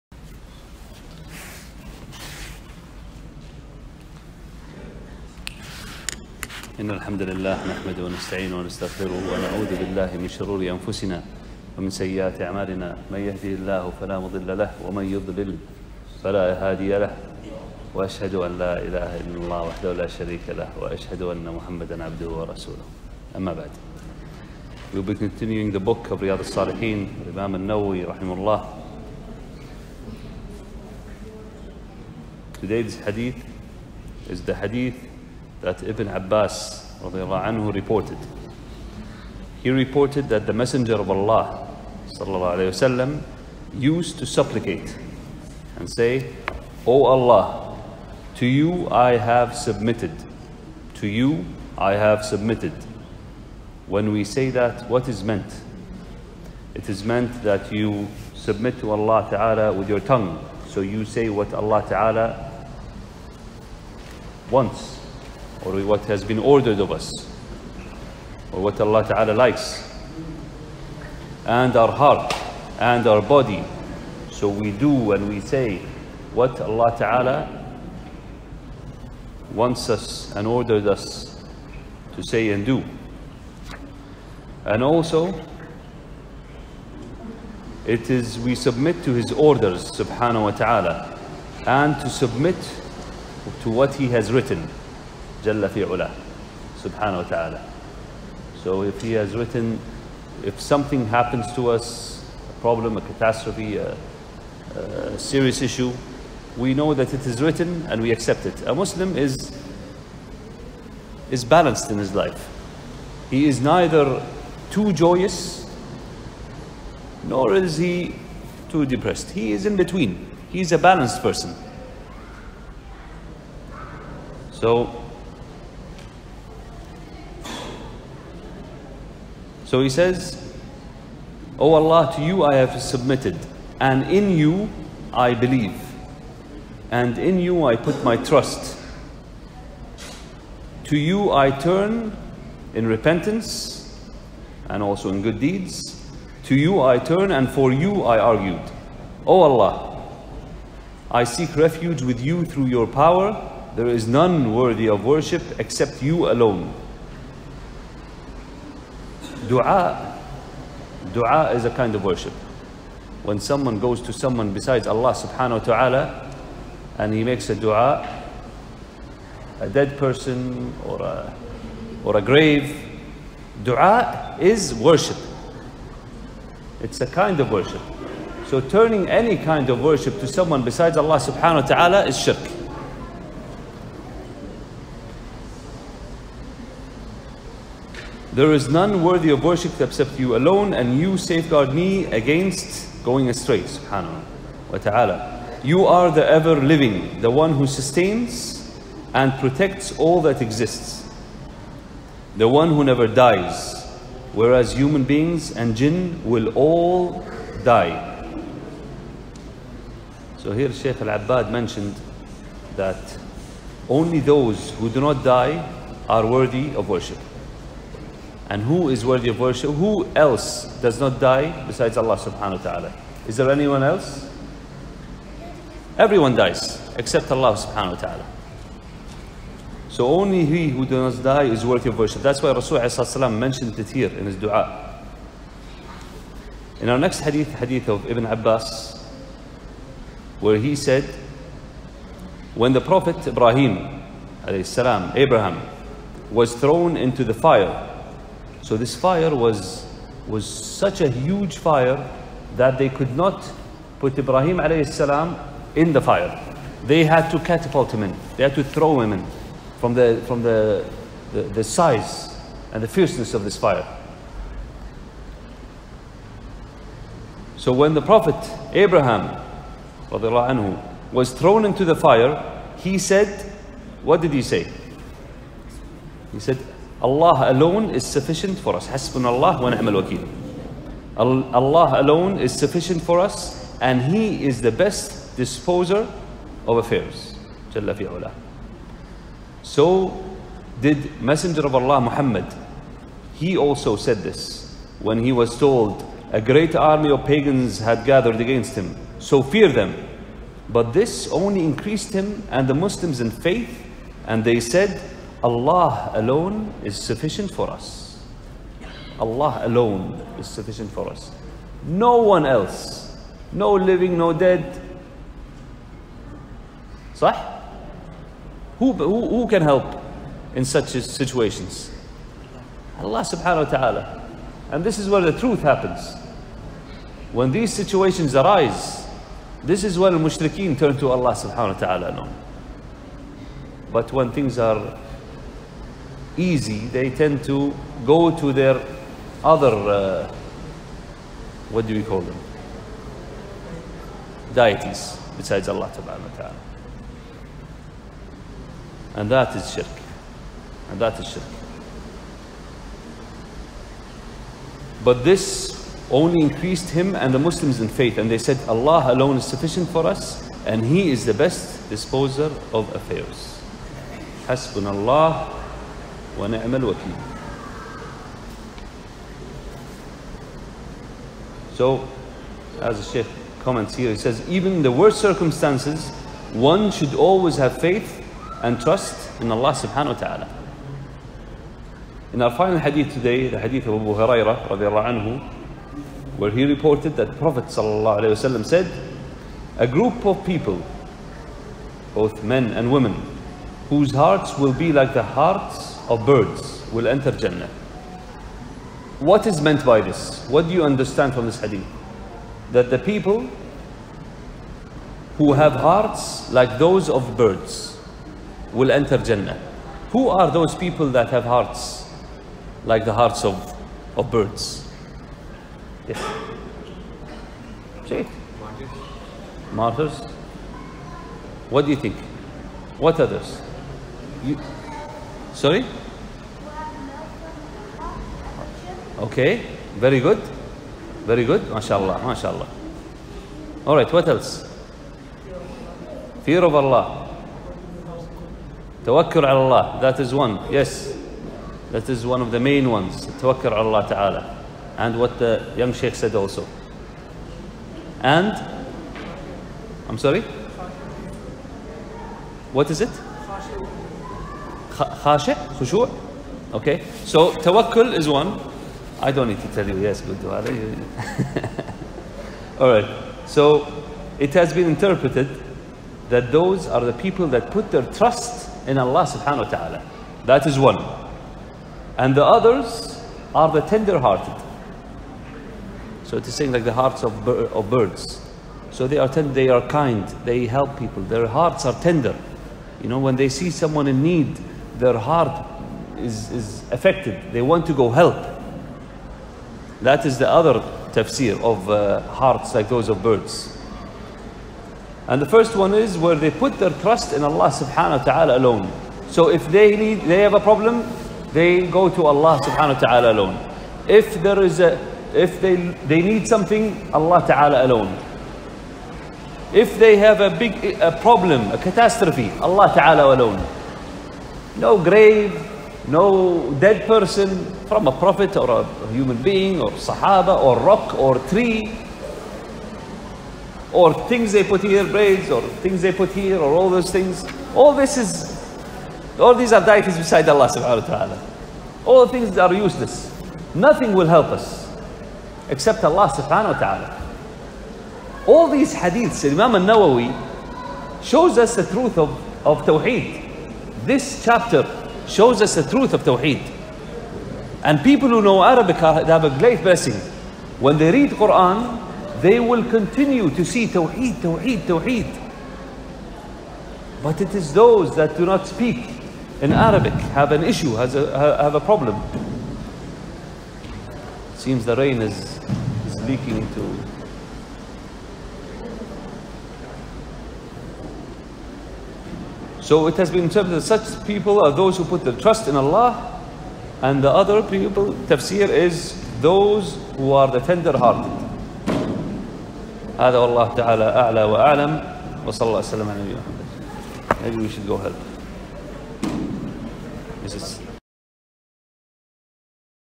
[Shaykh Ali Al-Hamad] Riyad as-Saliheen - Dars#25 - 161219
Lecture (Dars) #25... Explanation of Imam Nawawi's book "Riyad as-Saliheen - Gardens of the Pious" by Shaykh Ali Al-Hamad

About the Book:
An extremely popular and useful compilation of authentic hadith. Covering every aspect of Islamic belief and moral conduct, it selects approximately 2000 hadith from the six major collections: Bukhari, Muslim, Abu Dawood, Tirmidhi, An-Nisai and Ibn Majah.
Wide range of topics: sincerity of purpose, spending in the way of Allah, rules of fasting, seeking knowledge, attending funerals and safeguarding the Quran.
Riyadh as-Saliheen comprises of about 2000 ahadiths in 372 Chapters and 19 Sections.